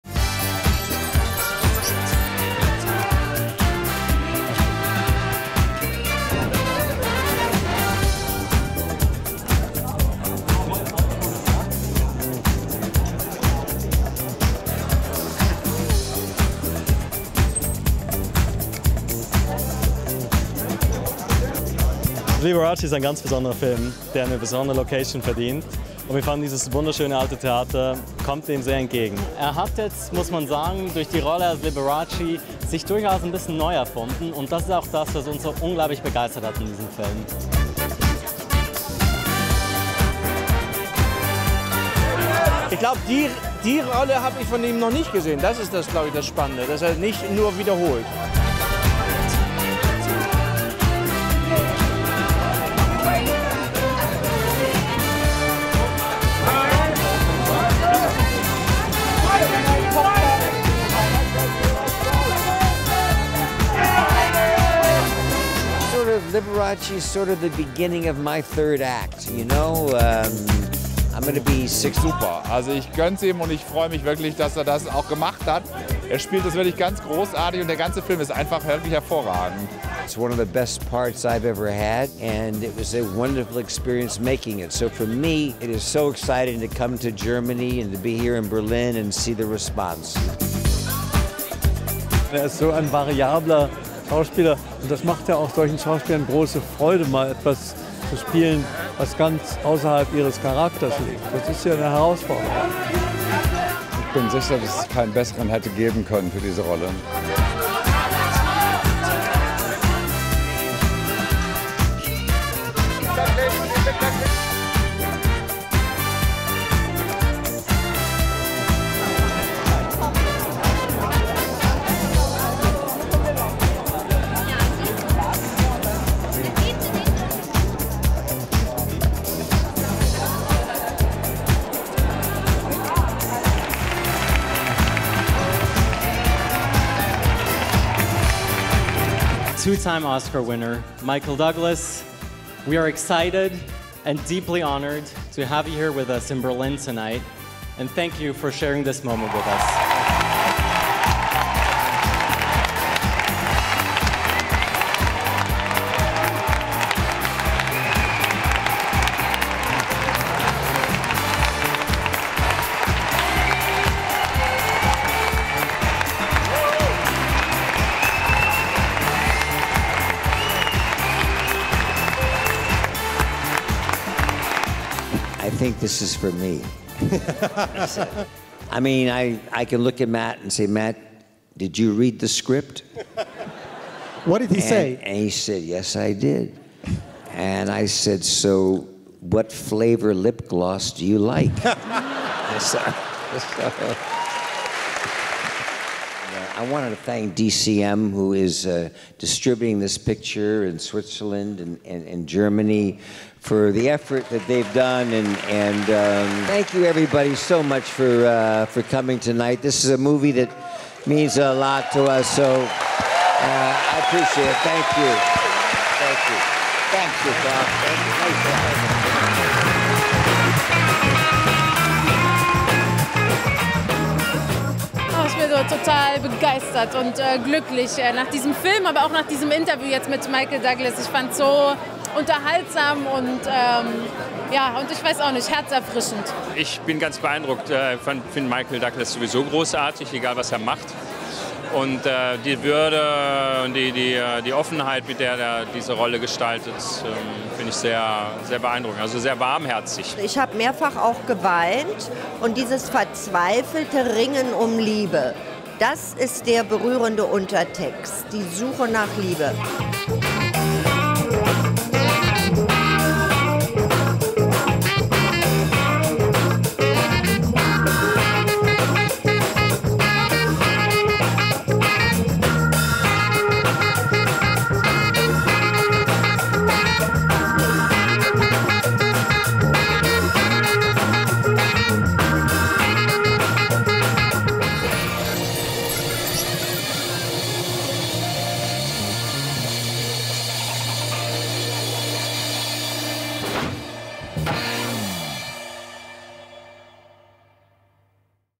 Michael Douglas im Gespräch mit ZEITmagazin-Chefreakteur Christoph Amend
Zur Premiere seines neuen Films "Liberace" kam Michael Douglas nach Berlin und sprach mit ZEITmagazin-Chefredakteur Christoph Amend.